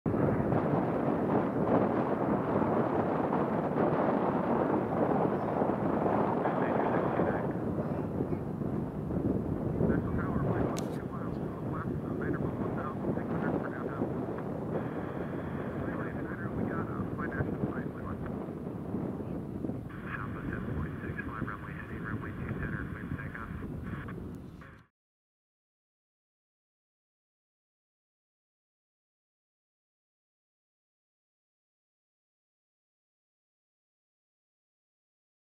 Benny Brown of Broken Bow Record's Private Jet
Benny Brown, founder of Broken Bow Records, flies away in his private jet. Broken Bow Records produced 21 number-1 hits for Jason Aldean.